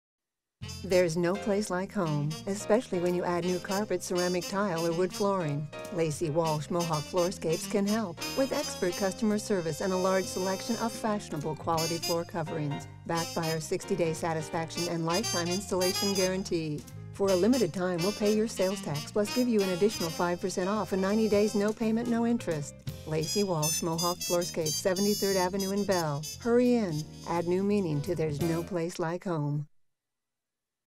Lacie Walsh Flooring TV Commercial Video Production in Arizona
Lacie Walsh Flooring Coop with Mohawk Carpet ad, TV commercial shot and edited by Anthony Miles of 4th Wall Productions, LLC High definition video production in Arizona Video production in arizona, HD, High Definition video, tv commercials, web video advertising